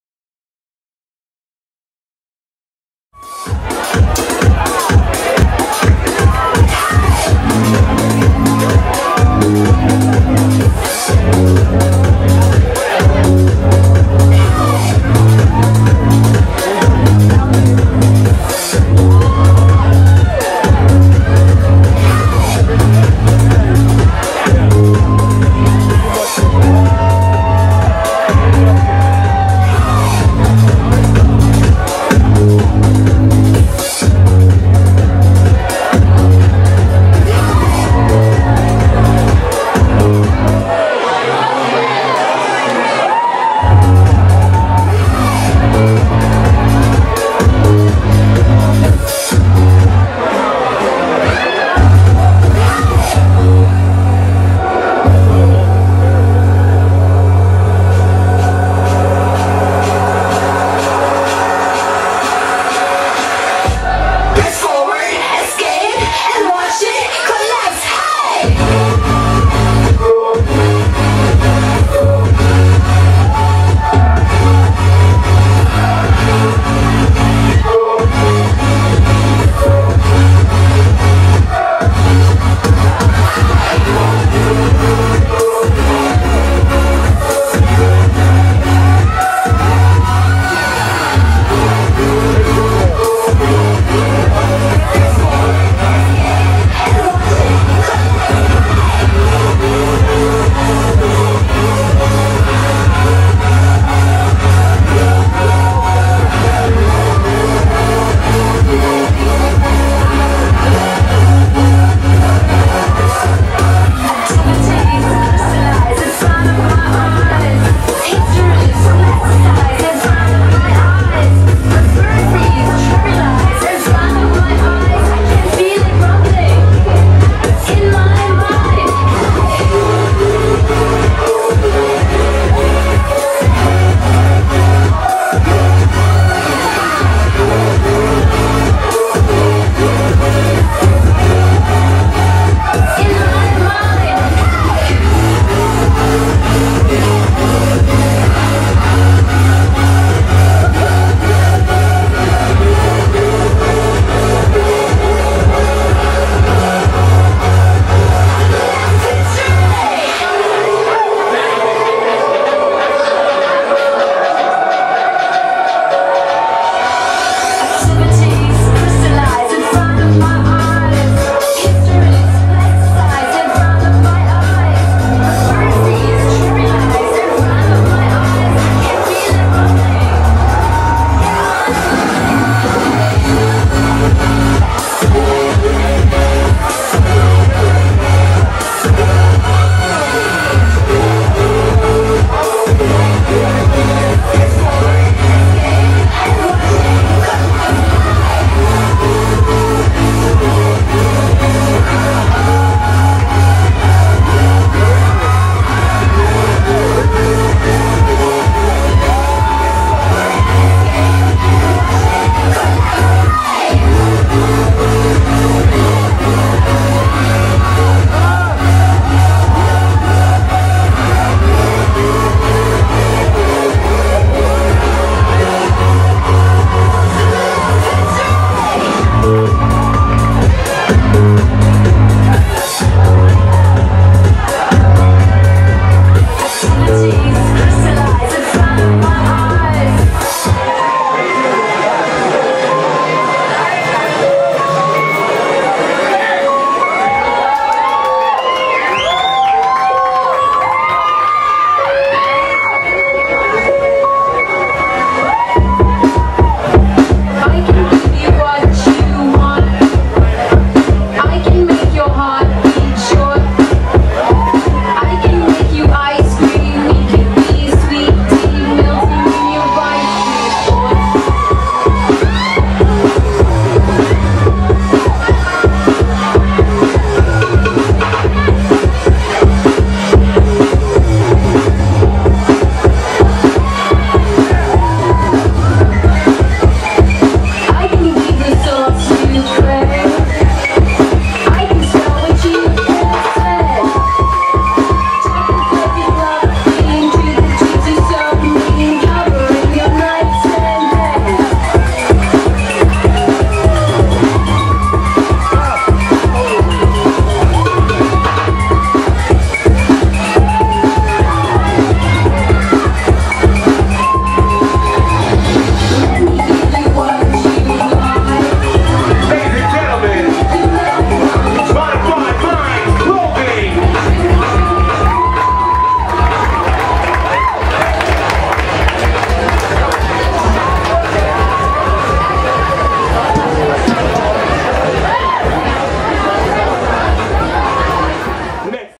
Jen Deveroux's Madame Trapeze 10 Modifiedminds Fashion Show 5/12/12 @ Crescent Ballroom
Models, in order of appearance: Lauren Perry, Cheryl Roden, Chelsea Padilla, Vanessa Jeanene, Kari Jones, Victoria Paege, Natasha DeMonterrozo and Casey Sponhouse/Cassandra Montez.  Ferris, designer, appears at the end of the finale walkthrough.

MUA: Jenasis Arts & various
Hair: Katt (Cassandra Montez) +

(video by my husband)